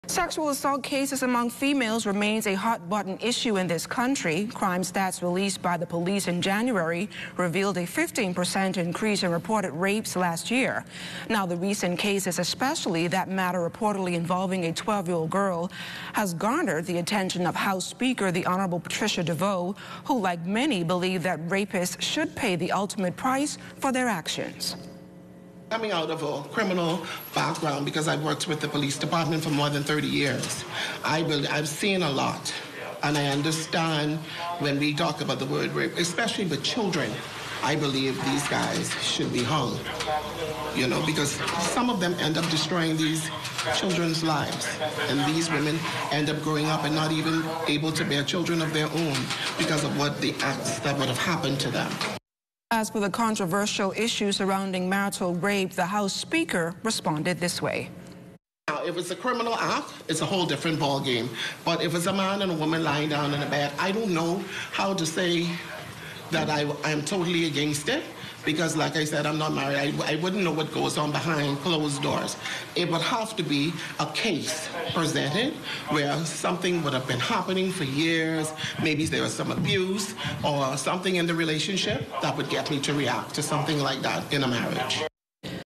HOA Speaker Weighs In On Teen Rape
Speaker of the House of Assembly, the Hon. Patricia Deveaux speaks on sexual assault against teens.